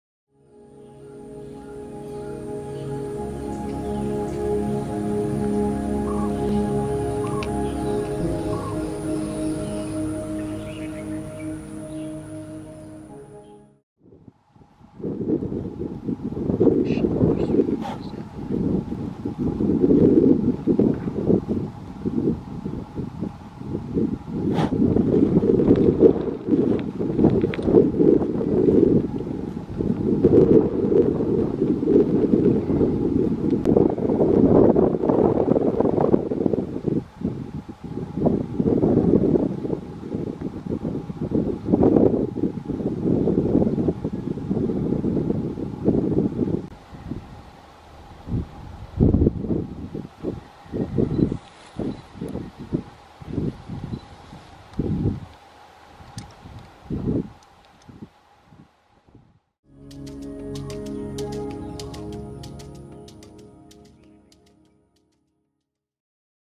20220830 TAPUITEN (NORTHERN WEATEAR)
Tijdens een van mijn vogelroutes zag ik plotseling zes (!) tapuiten op meerdere paaltjes zitten. En dan is het haastje repje om vooral rustig te blijven maar toch ook foto's en filmpjes te kunnen maken.
Dit tafereel zag ik bij Kasteelhoeve Hartelstein in natuurgebied Rivierpark Maasvallei bij Itteren-Maastricht.